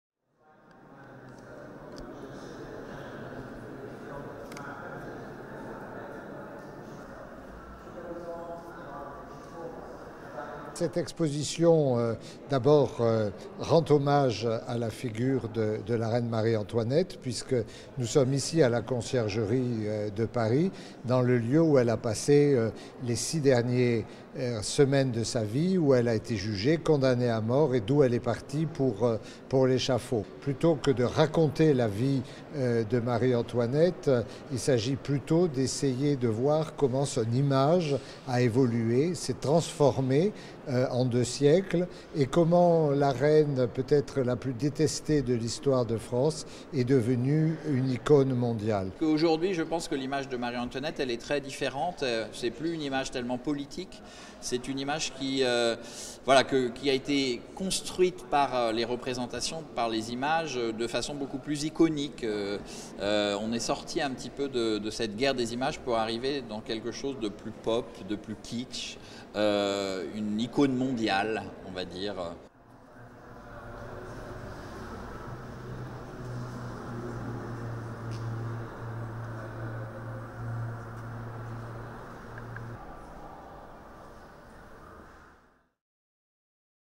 Marie-Antoinette, a popular icon explored through contemporary culture. | AFP
Marie-Antoinette may have lived centuries ago but she remains a fascinating figure in popular culture. Through cinema, contemporary art and the world of fashion, a new exhibition at the Conciergerie in Paris explores the many-layered representations of France's last queen. "Marie-Antoinette, metamorphoses d'une image" runs from 16 October 2019 to 26 January 2020.